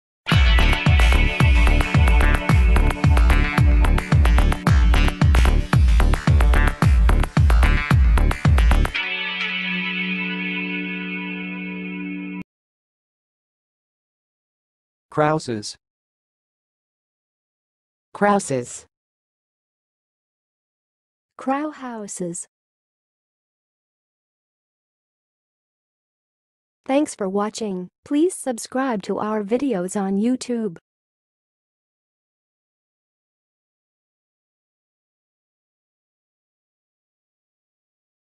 How to say "krause's"! (High Quality Voices)
Watch in this video how to say and pronounce "krause's"!